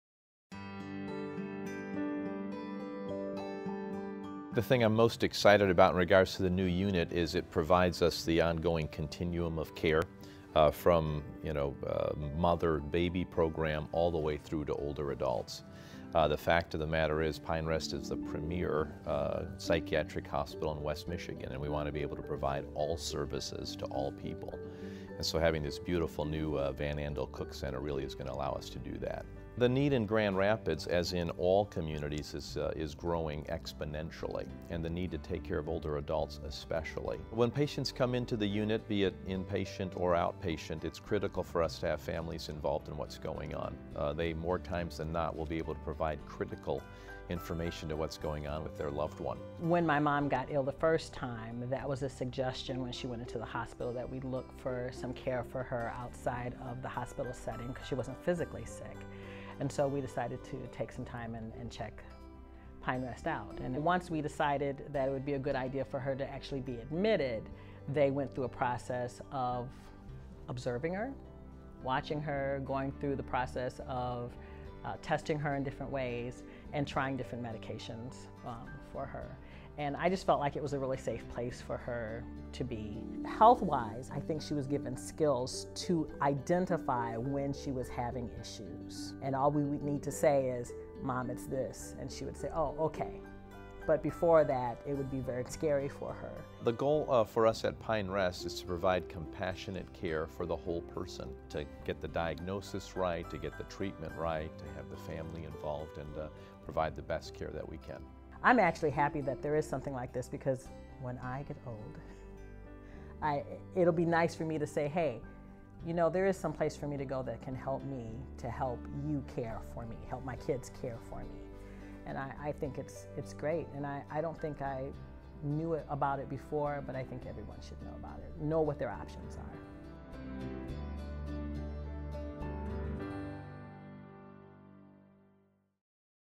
Older Adult Services at Pine Rest
Video from the 2013 Pine Rest Foundation Dinner visits the new older adult unit at Pine Rest, Dr. Jack Mahdasian and the daughter of a patient.

JACK MAHDASIAN, MD, MA
SENIOR PSYCHIATRIST FOR OLDER ADULTS
Having a particular interest in older adults, Dr. Mahdasian, Senior Psychiatrist for the Van Andel • Cook Center for Dementia & Geriatric Behavioral Health, is passionate about the treatment of behavioral health concerns and innovative research for this vulnerable group.